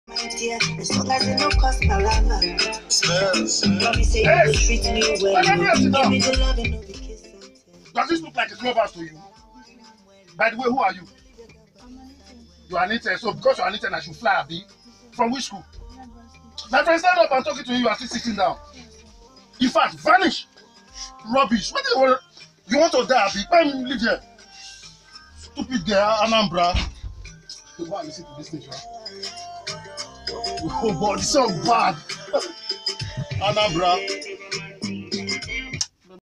Say No To Office Bullying
This video condemns any form of bullying irrespective of your Position